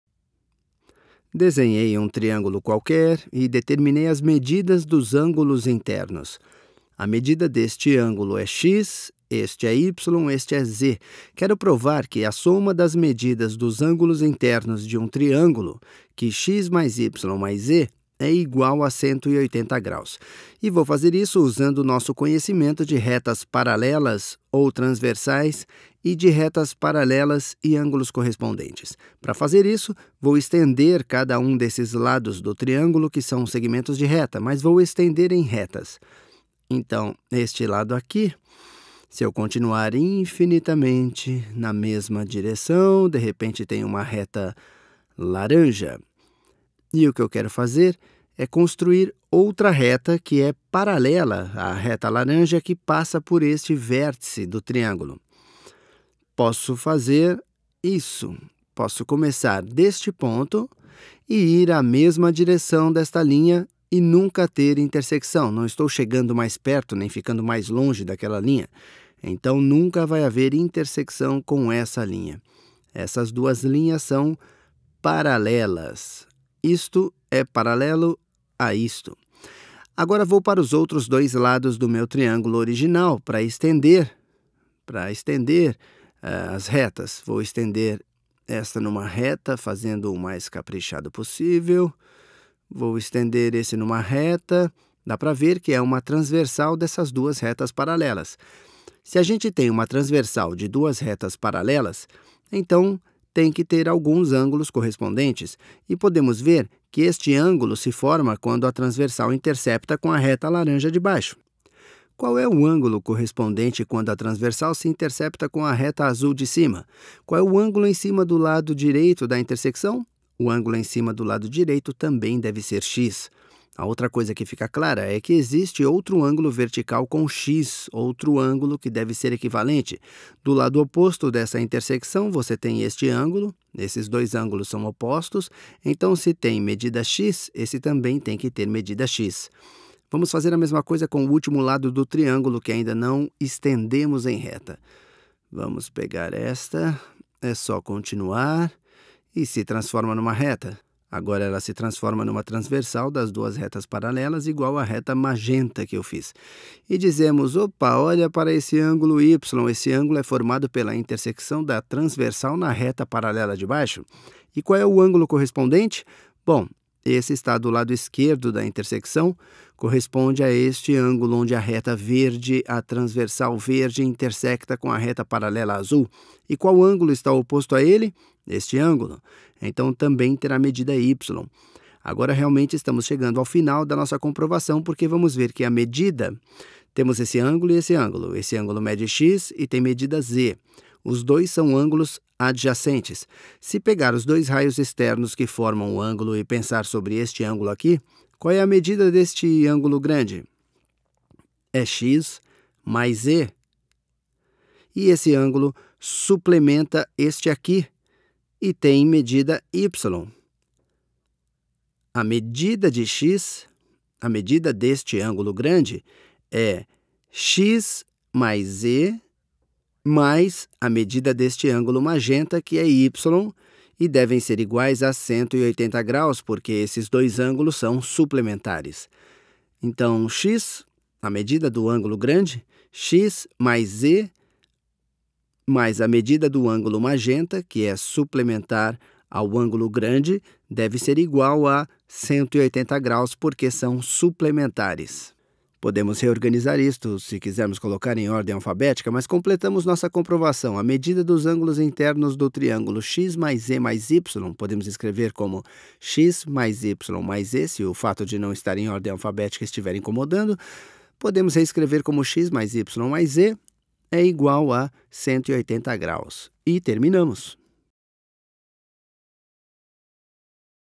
Demonstração: a soma das medidas dos ângulos de um triângulo é 180
Vídeo sobre demonstração da soma das medidas dos ângulos internos de um triângulo
Titulo em inglês: Angles and intersecting lines